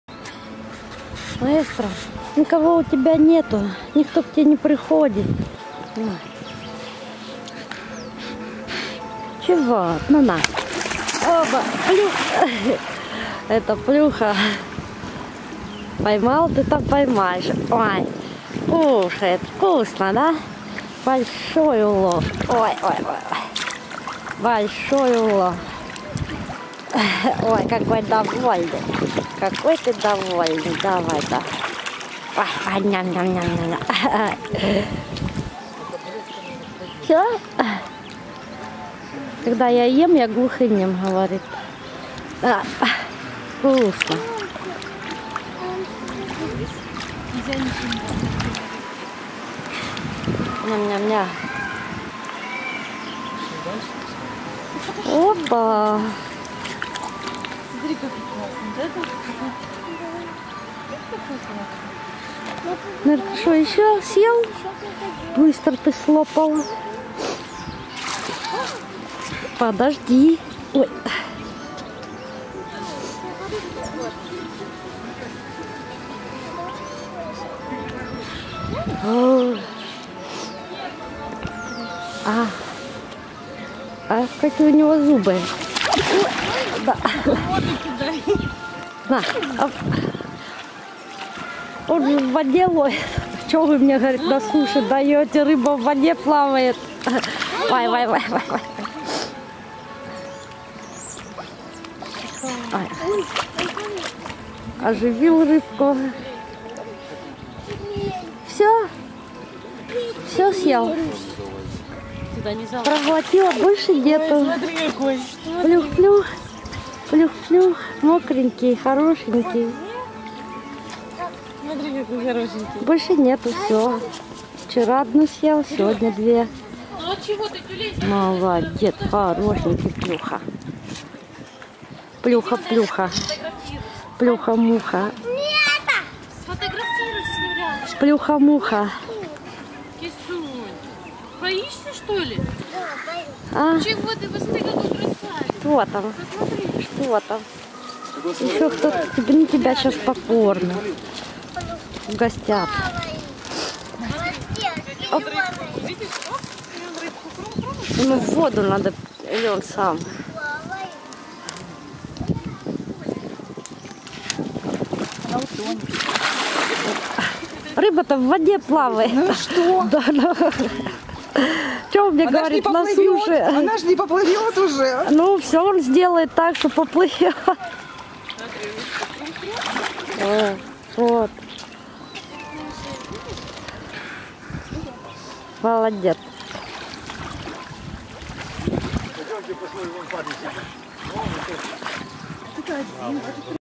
Маэстро с большим уловом))) Тайган. Белогорск. Крым.
У Маэстро такая отчётливая мимика и выражение взгляда, что без ошибки можно определить его желание или настроение! Минтай по душе ему пришёлся и половил и поел!!!